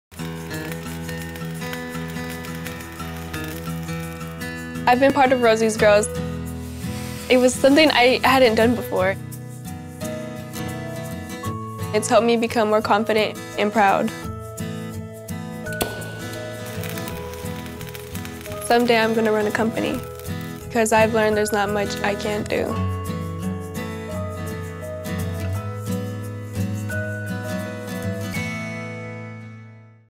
Rosie's Girls: where confident girls grow into powerful women
At Chevron, we know confident girls grow into powerful women. That’s why we support Rosie’s Girls Richmond.
(30 seconds)